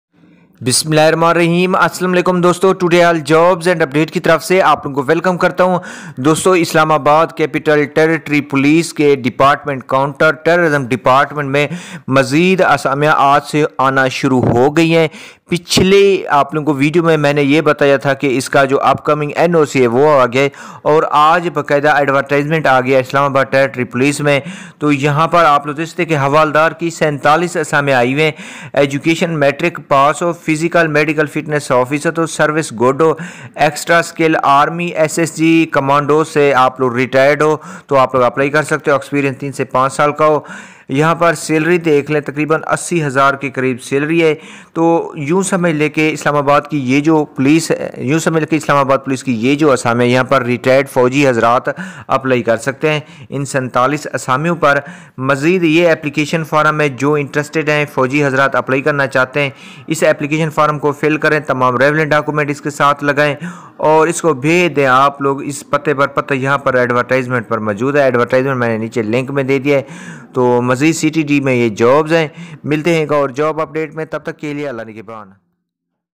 ctd police jobs in ICT Islamabad capital territory Police 2021
General InstructionsS
1. Interested candidates having above mentioned qualifications, experience and falling within the age limit should send
their applications on prescribed Performa to SSP/Counter Terrorism Department (CTD Complex Sector H-11,
Islamabad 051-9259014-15) within fifteen (15) days of publication of this advertisement.
2. Retirement period of the candidates from Pak Army/Navy/PAF should not be more than 03 years before joining CTD
on contract basis...

آسلام آباد پولیس سپائی نئی بھرتی 👇

No relaxation in age whatsoever is permissible.
4. Call letters will be issued to the short listed candidates only.
5. Above mentioned qualification is given as minimum, additional qualification in the relevant field will be preferred.
6. Applicants who do not fulfill minimum qualification/experience, requirements/standard may not apply.
7. Applications received late or incomplete will not be entertained.
8. Departmental Selection Committee will select the eligible candidates and its decision in this regard shall be final.
9. Qualified candidates will have to go through complete medical examination by the Medical Board/Civil Surgeon.
10. No TADA will be given to candidates for appearing in testinterview or at the time of joining.
11. The selected candidates will be paid lump-sum salary as mentioned above.
12. or contact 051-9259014, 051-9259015.
(DR. NAVEED ATIF) PSP
Assistant Inspector General of Police (Establishment
PID)1259/21/C
Islamabad

ICT Islamabad capital territory Police jobs

کوئی مسلہ تو مسیج کریں 👇
Fallow me on Instagram: